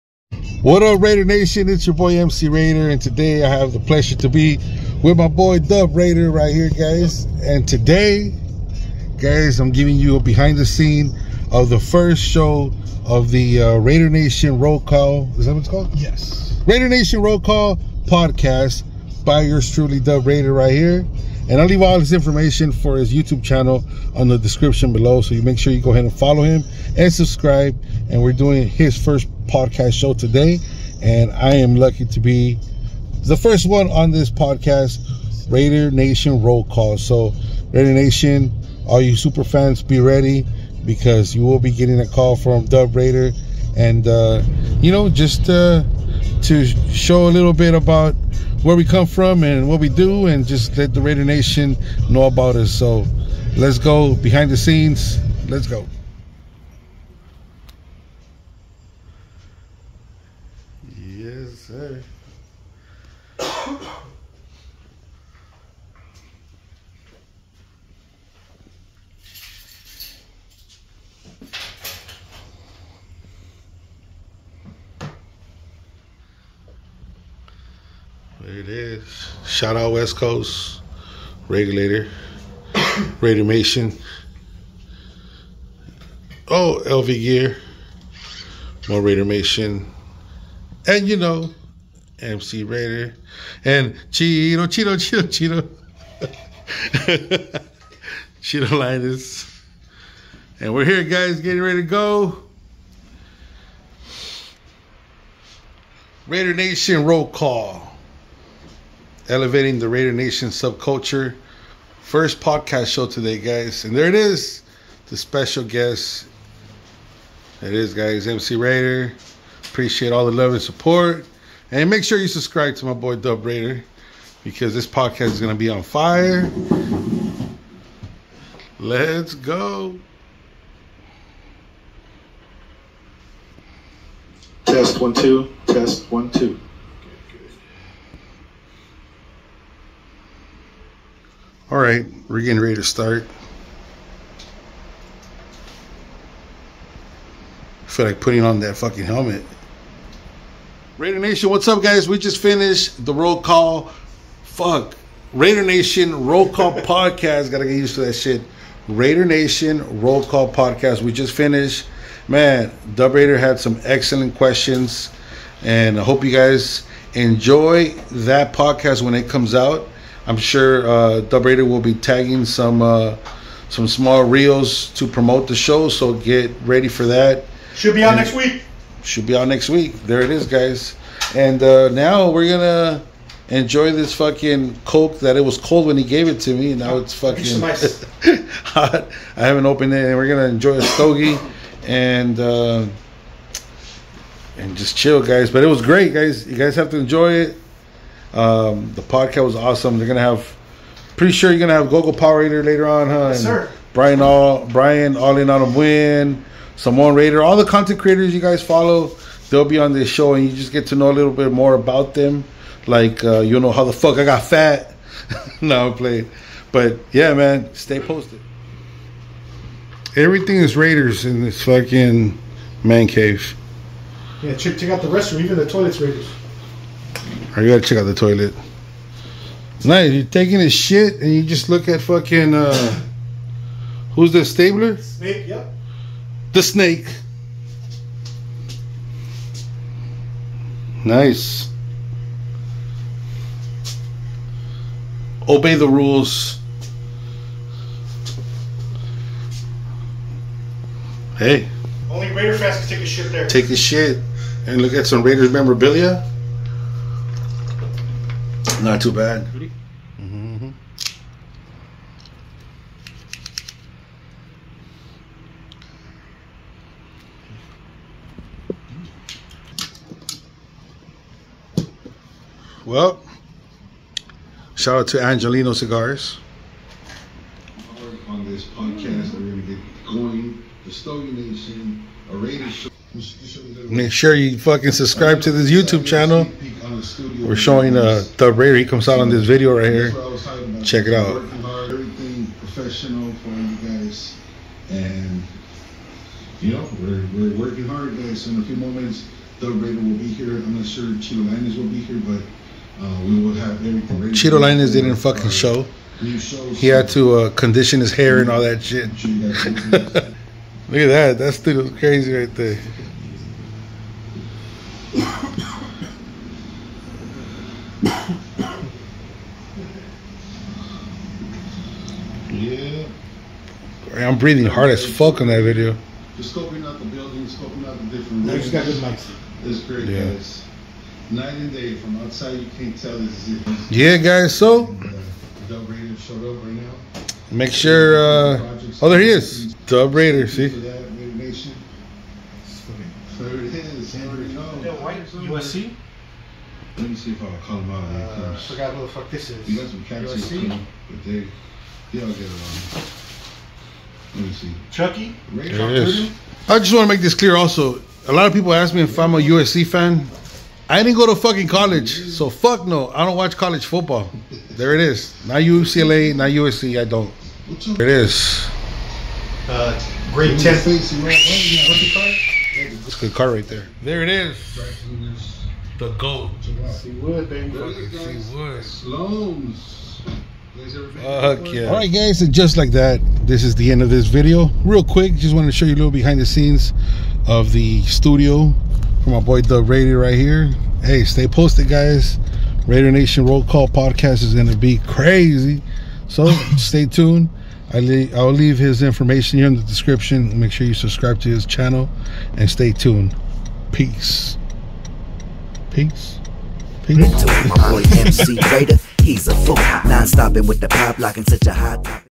Behind the scenes at Dub Raiders studio
Today, I had the honor to be on the Dub Raider podcast show "Raidernation Roll Call Podcast."Here is a little video from behind the scenes .